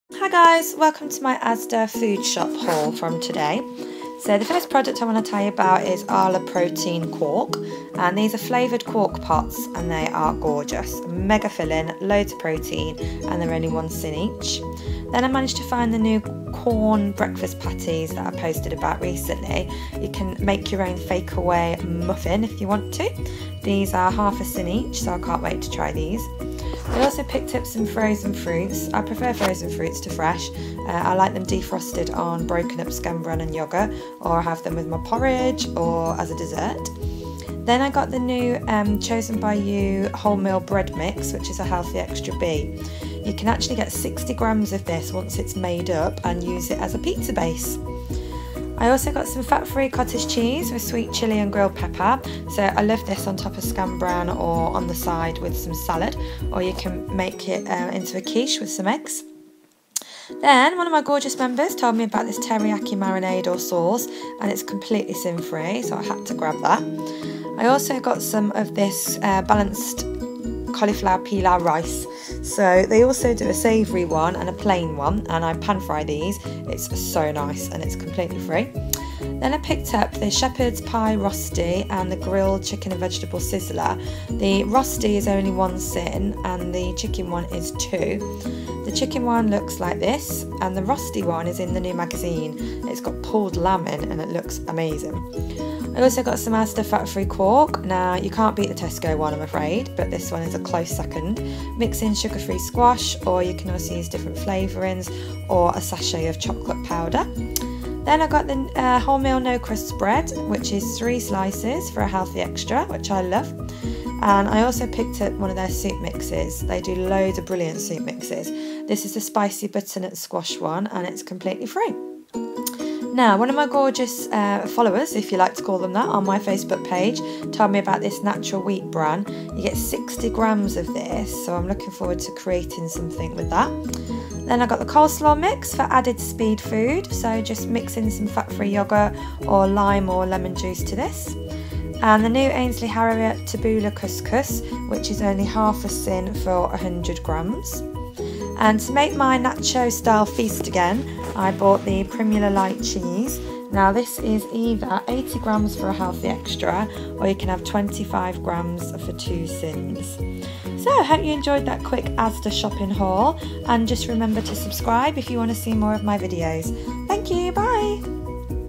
***Slimming World Asda food haul***
Loads of fantastic things I picked up from Asda today x